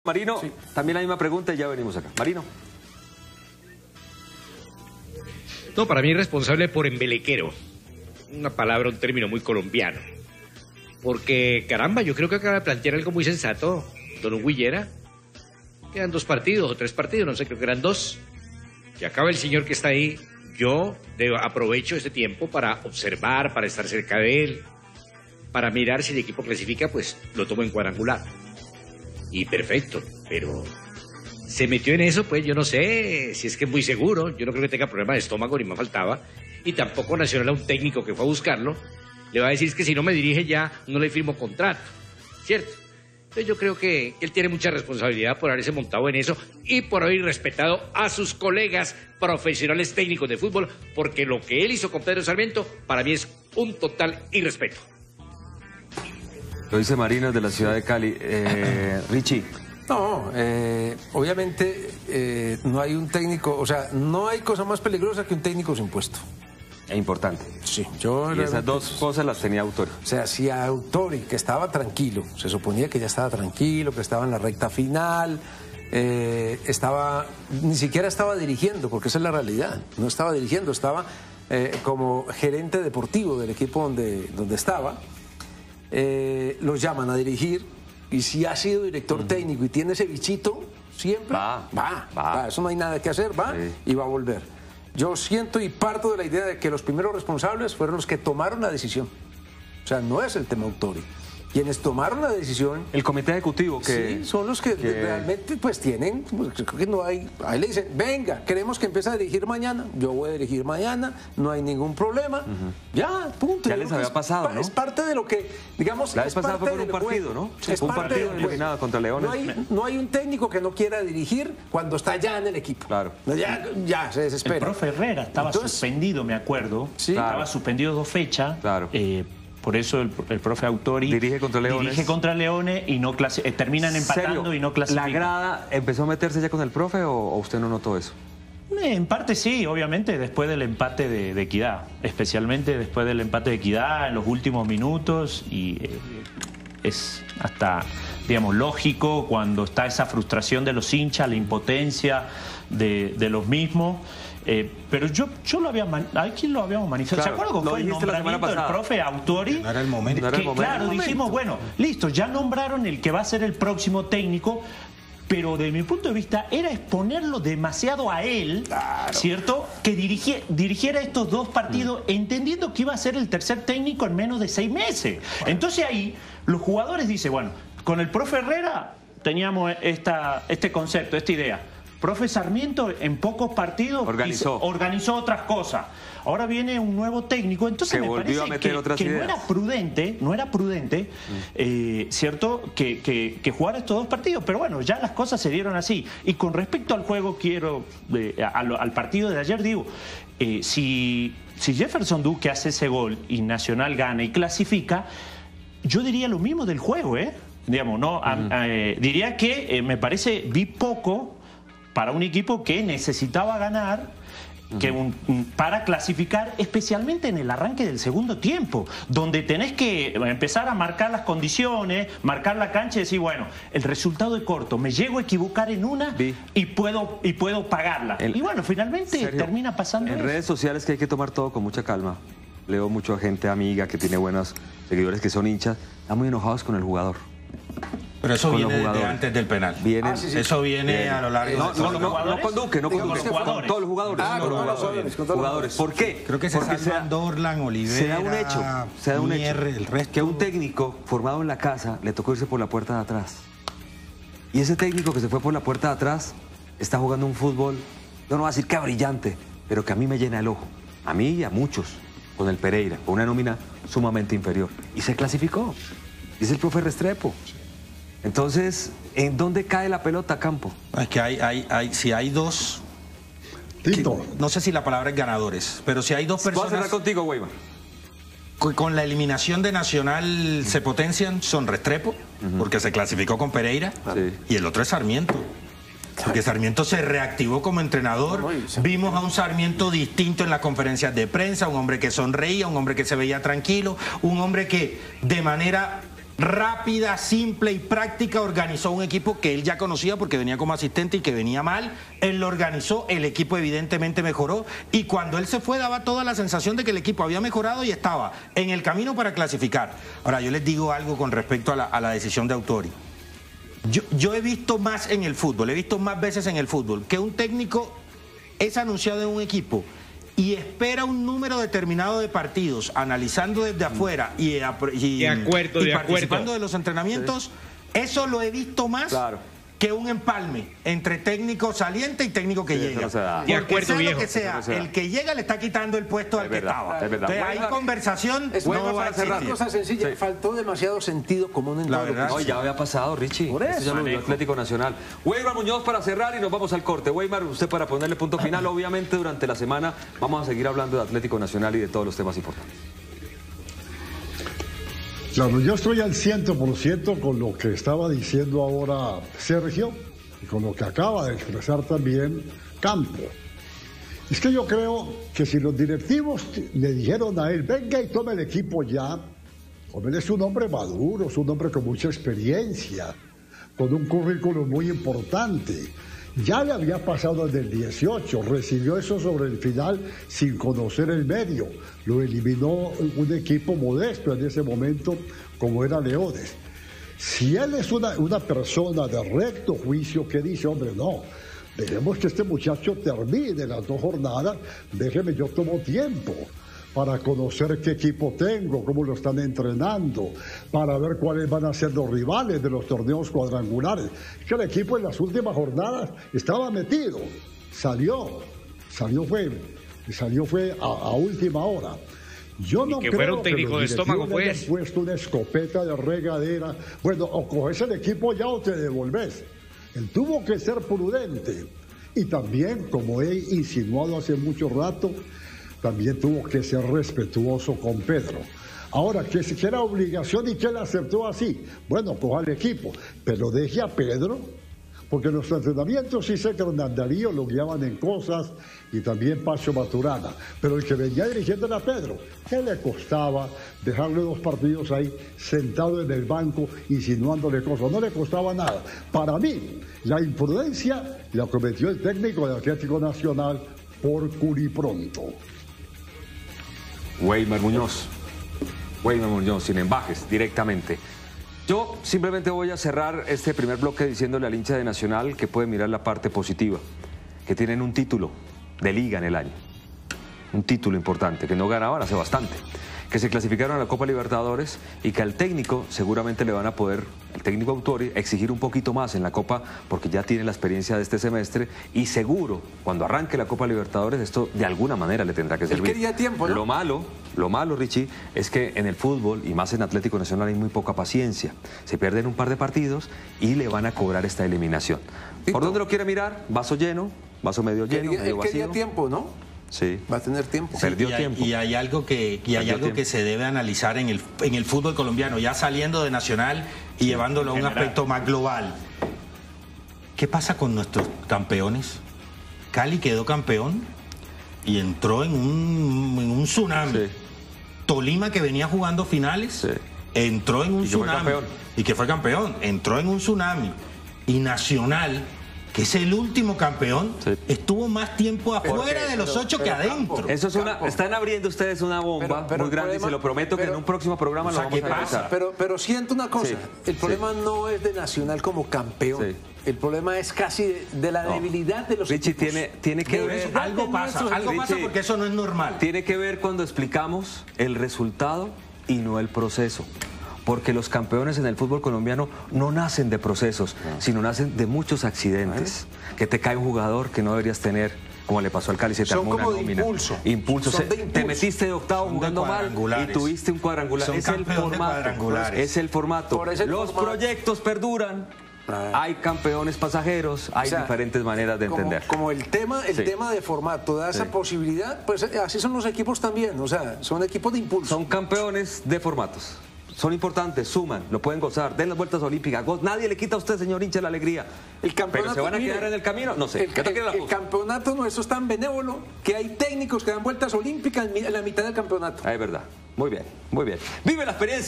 "Autuori debió ser respetuoso con Pedro y haber conocido más a Nacional" Wbeimar Muñoz en Conexión
El panelista indicó que el técnico brasileño debió esperar un poco más antes de tomar el equipo..